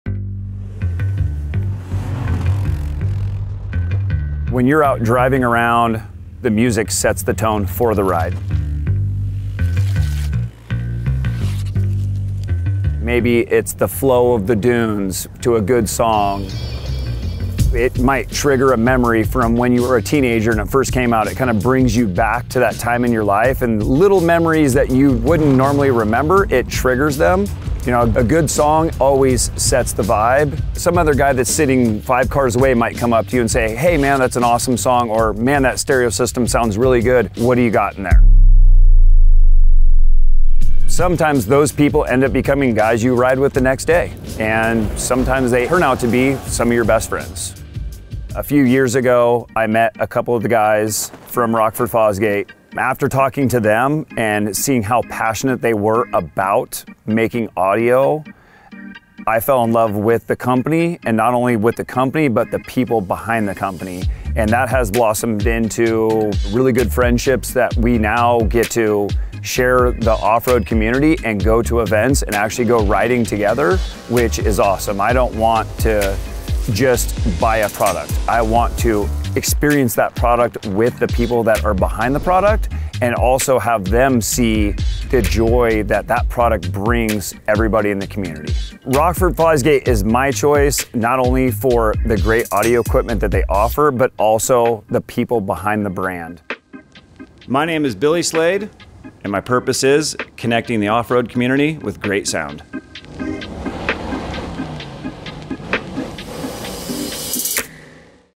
The Defined: Billy Slade | E09
Meet Billy Slade – an off-road enthusiast and the ultimate Rockford Fosgate Side by Side fanatic. Check out this episode to learn more about his passion and purpose.